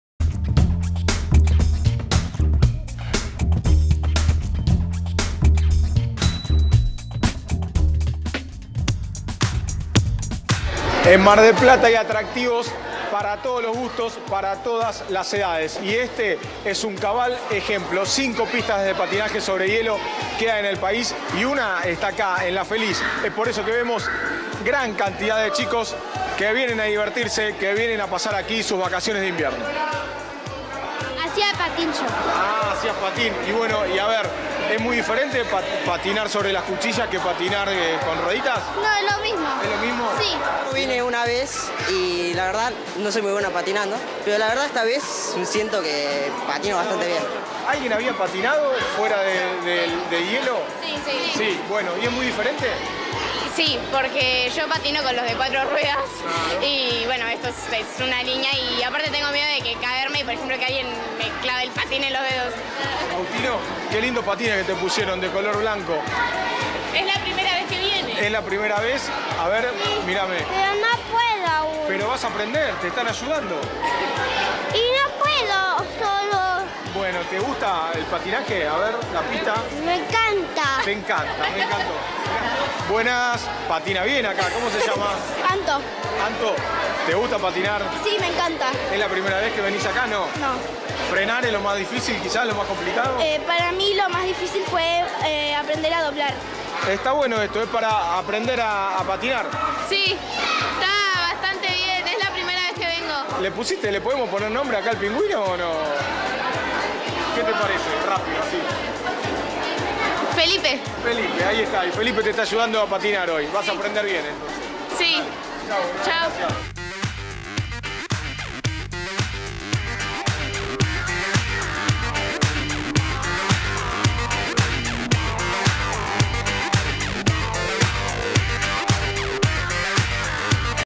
La Feliz, en patines
La ciudad de Mar del Plata ofrece una alternativa sobre ruedas para disfrutar de las vacaciones de invierno. El testimonio de niños, niñas y adolescentes recogido por los enviados especiales del noticiero de la Televisión Pública.